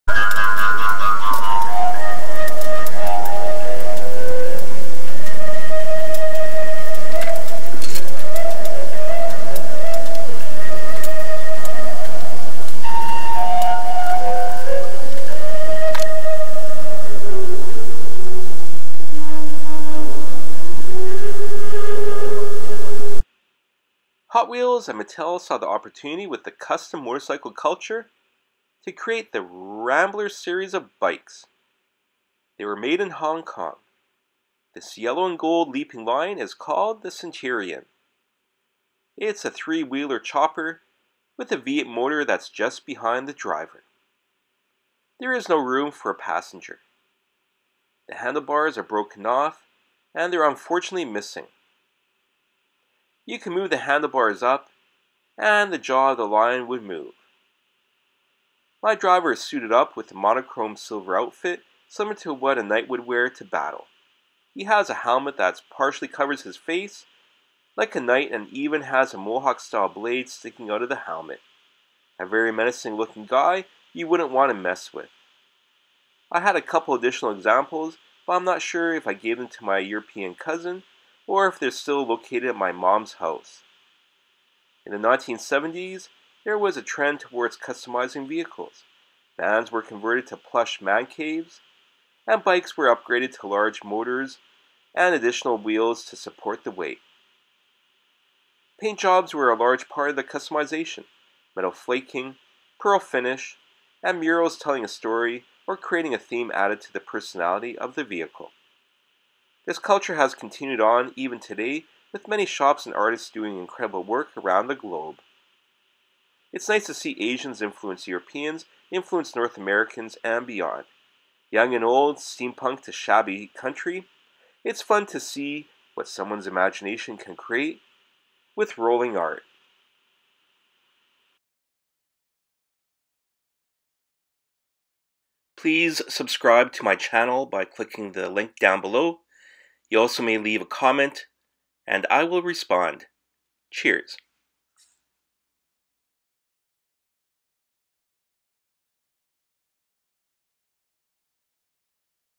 rrrumblers Centurion by Mattel
Hot Wheels and Mattel created this new group of custom motorcycles in the 70's.